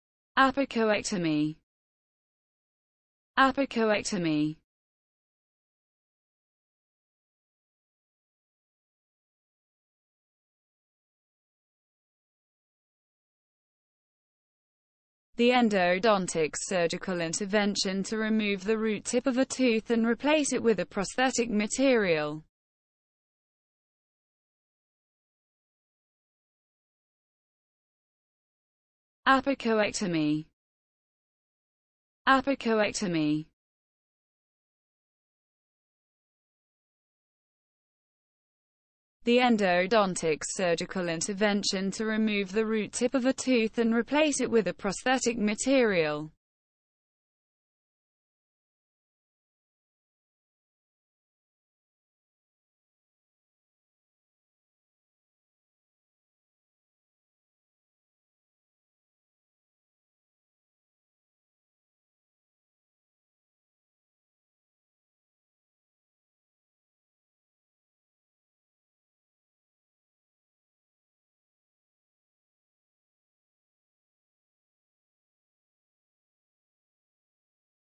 What does Apicectomy mean?
What does Apicectomy mean in English? Learn the meaning of the word Apicectomy! How to Pronounce Apicectomy. Expand your vocabulary, learn words.